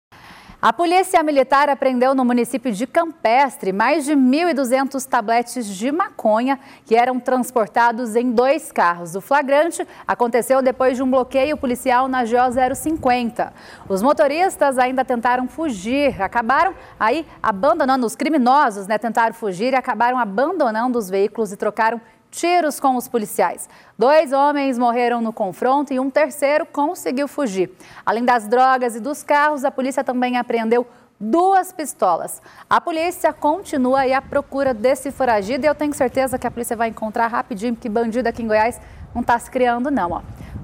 POLÍCIA MILITAR APREENDE 1200 TABLETES DE MACONHA EM CAMPESTRE
Apresentadora: Danuza Azevedo